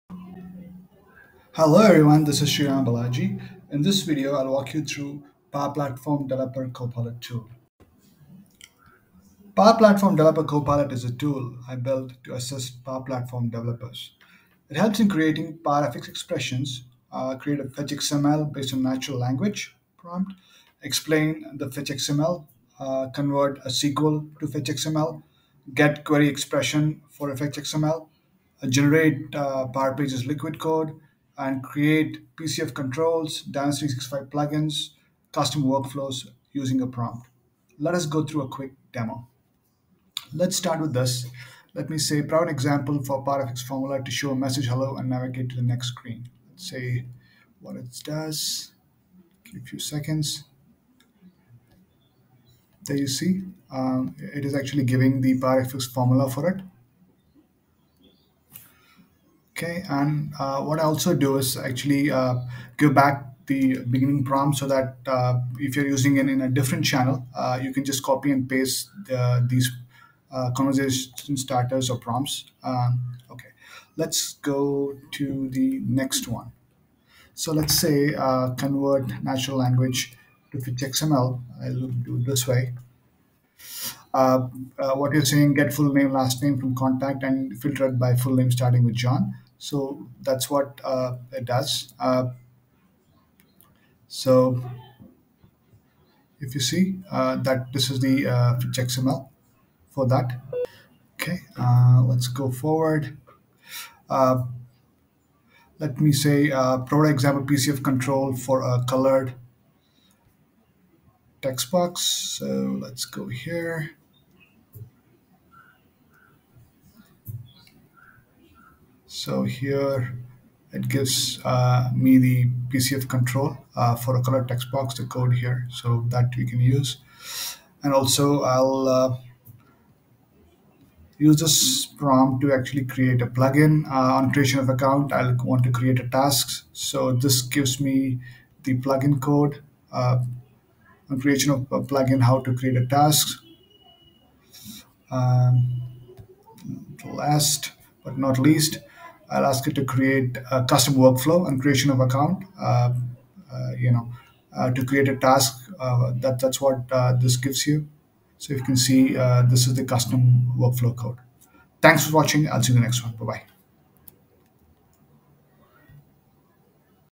Power Platform Developer Copilot Tool
Power Platform Developer Copilot is a  tool to assist Power Platform developers. It helps in creating PowerFX expresssions, create FetchXML based on natural language, explain FetchXML, convert SQL to FetchXML, get queryexpresssion for FetchXML, generate Liquid code and create PCF Controls, Dynamics 365 Plugins,  custom workflows using a prompt.